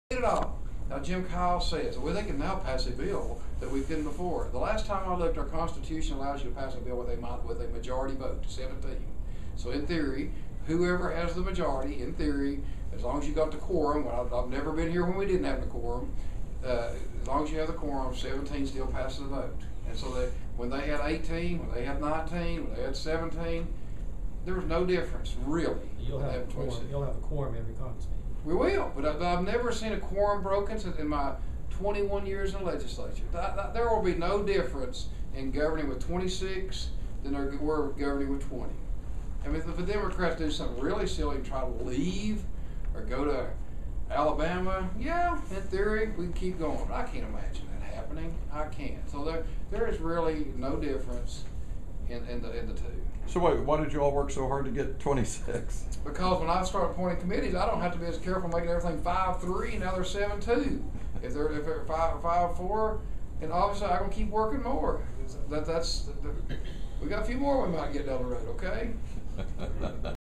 Ramsey: I've Never Seen a Quorum Broken (TNReport.com)
Tennessee Lt. Gov. Ron Ramsey, R-Blountville, explains his feelings on the accusations from Sen. Jim Kyle, D-Memphis, that the GOP can now pass bills they couldn't pass before they had a Supermajority.

"The last time I looked, our constitution allows you to pass a bill with a majority vote -- 17.  So, in theory, whoever has the majority, in theory, as long as you go up to quorum -- and I've never been here when we didn't have a quorum -- as long as you have the quorum, 17 still passes a vote. When they had 18, when they had 19, when they had 17, there was no difference, really."